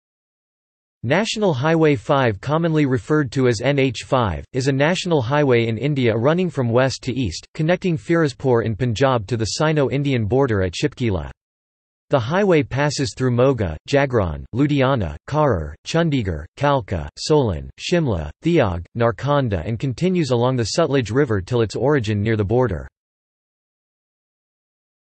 National Highway 5 (India) | Wikipedia audio article
This is an audio version of the Wikipedia Article:
National Highway 5 (India)

- Socrates

SUMMARY
National Highway 5 commonly referred to as NH5, is a national highway in India running from West to East, connecting Firozpur in Punjab to the Sino-Indian border at Shipki La. The highway passes through Moga, Jagraon, Ludhiana, Kharar, Chandigarh, Kalka, Solan, Shimla, Theog, Narkanda and continues along the Sutlej River till its origin near the border.